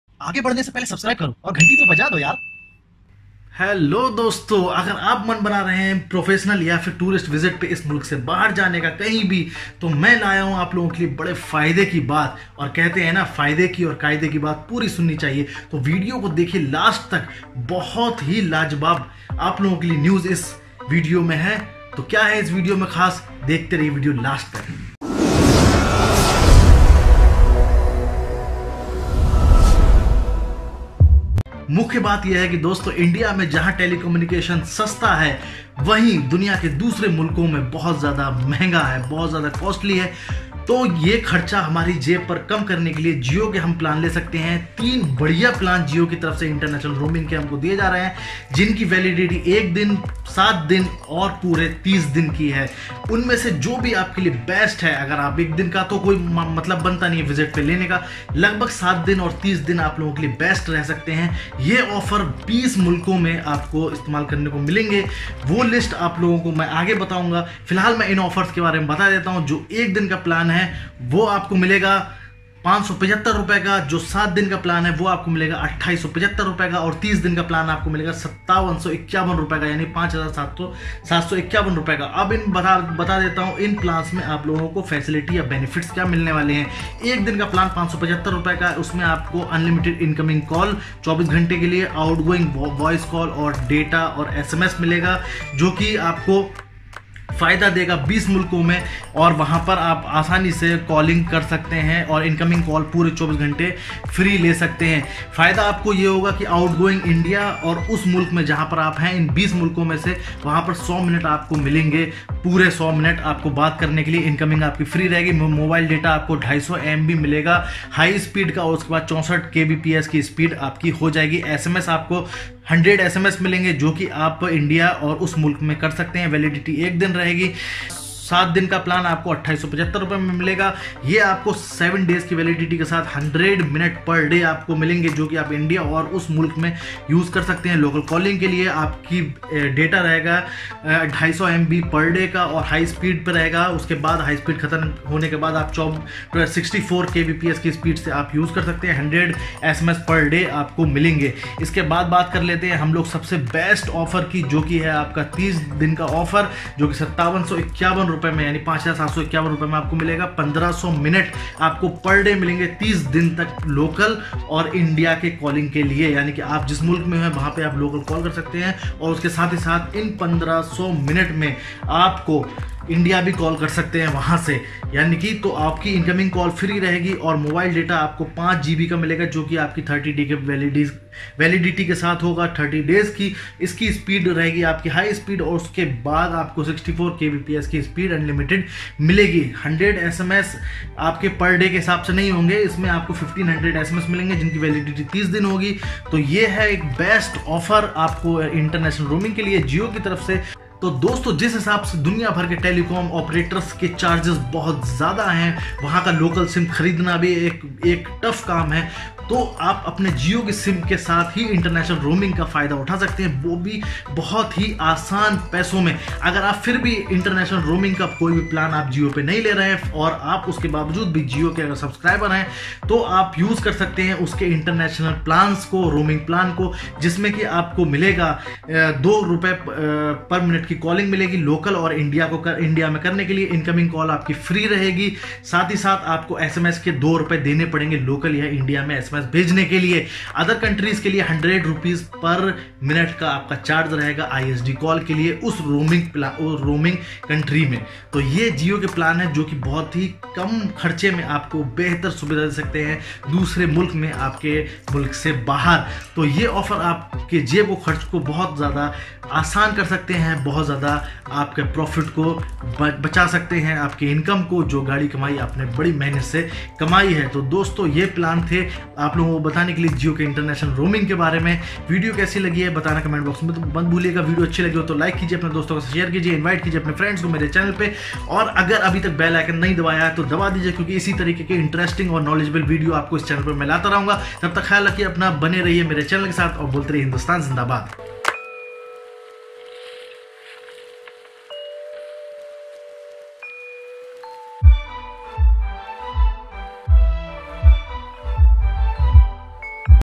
Jio | Best International Roaming Plans | Jio Mobile
Hello Friends,
Dosto agar aap plan bana rahe kahin bahar jane ka to ho jaiye tayar ho select kijiye your best international roaming offers Jio se.